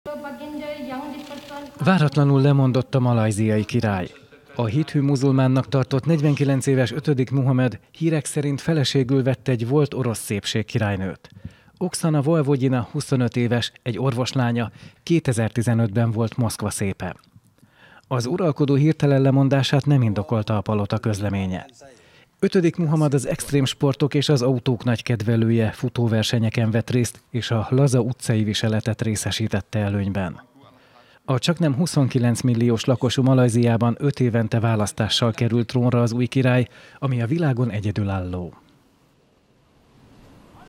Váratlanul lemondott a malajziai király
25 éves orosz szépségkirálynőt vett el a 49 éves malajziai király.…